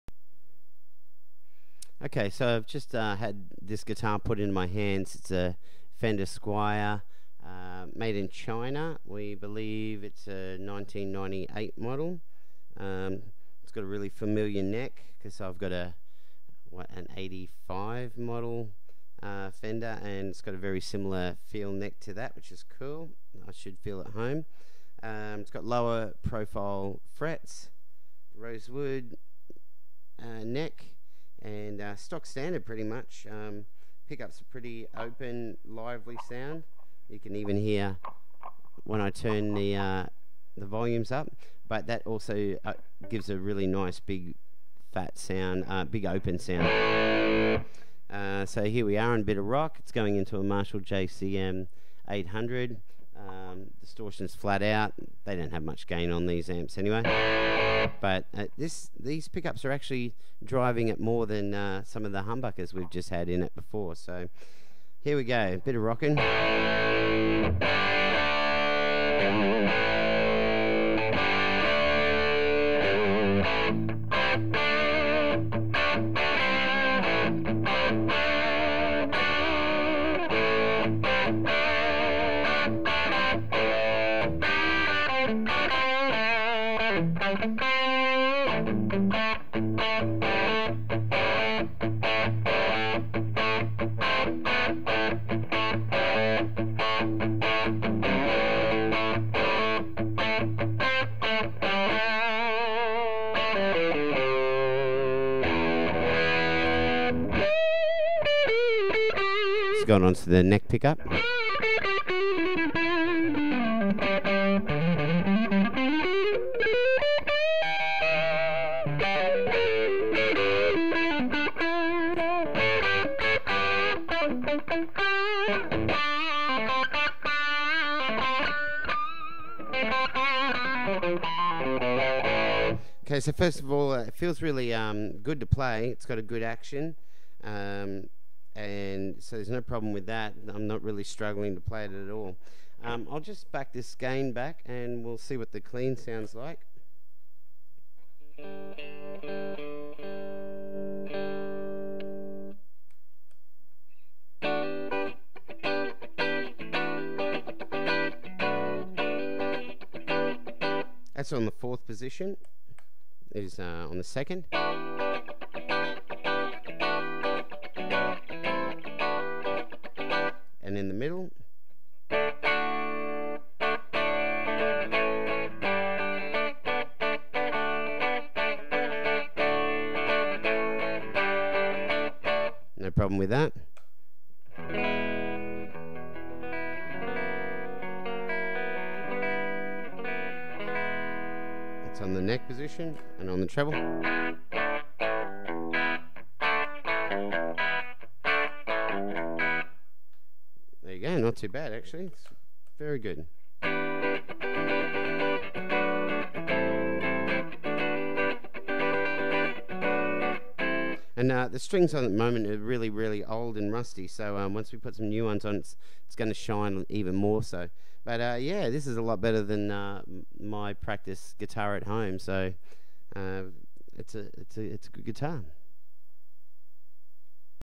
98 CY Squire Affinity Stratocaster trough a Marshall JCM 800
Check out the review on the web site, click the link above.

A very cool budget guitar that is definitely worth considering. A few mods will bring this guitar up to a very playable standard for any guitarist.

If you see one of these older CY serial number Squires come up and you need a good back up guitar then go for it.